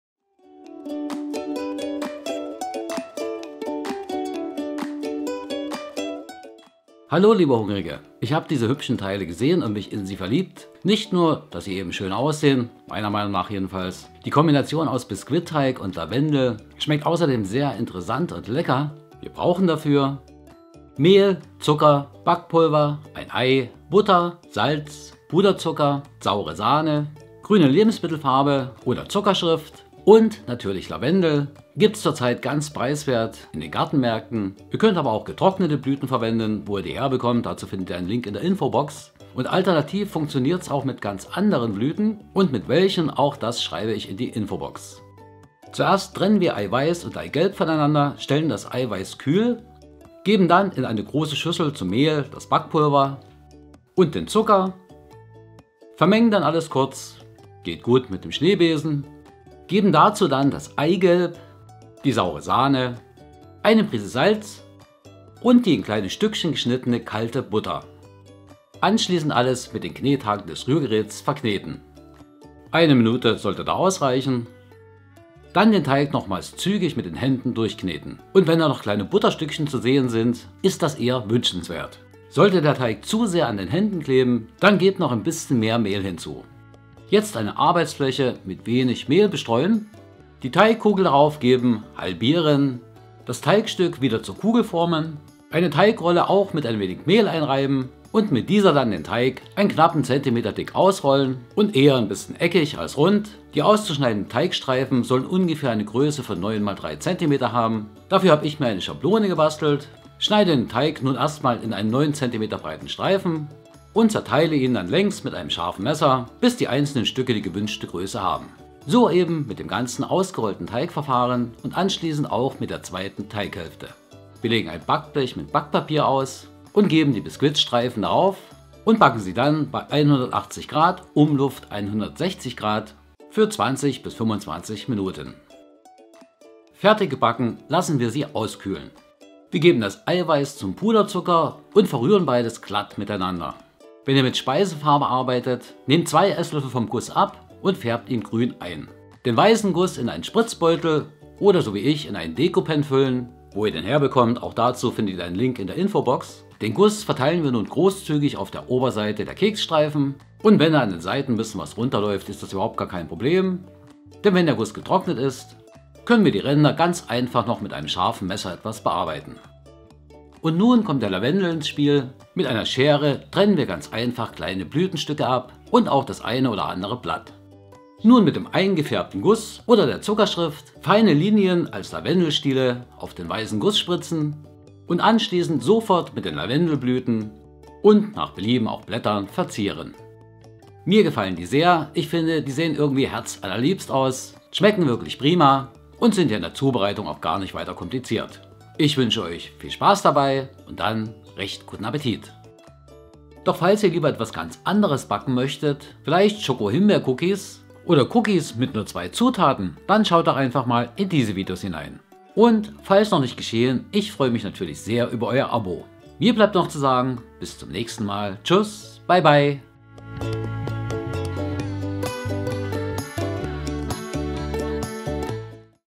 LAVENDEL-BISKUITSTREIFEN
Diese Biskuitstreifen sind nicht nur hübsch anzuschauen. Lavendel und ein lockerer Biskuitteig - das ergibt auch eine außergewöhnliche und sehr leckere Geschmackskombination.
► Lavendelblüten gibt es z. B. hier bei Amazon
►Den Deco Pen gibt es z. B. hier bei Amazon

ZUTATEN für ca. 20 Stück:
• 350 g Mehl
• 200 g saure Sahne
• 140 g Zucker
• 1 Ei, mittelgroß
• 1 Prise Salz
• 125 g kalte Butter, gewürfelt
• 1/2 TL Backpulver
• ca. 250 g Puderzucker
• Lavendel, frisch oder getrocknet
• grüne Lebensmittelfarbe oder Zuckerschrift
Hinweis: Sollte die Glasur zu dickflüssig sein, dann zusätzlich 1 bis 2 EL Milch einrühren
Das Biskuitstreifen bei 180 Grad, Umluft 160 Grad, für 20 bis 25 Minuten backen.

Diese Blüten sind essbar (allerdings übernehme ich keine Verantwortung dafür, da ich selbst noch nicht alle getestet habe):

Nach einer Rezeptidee aus „Lecker Einladen“, 4/2016, S. 35.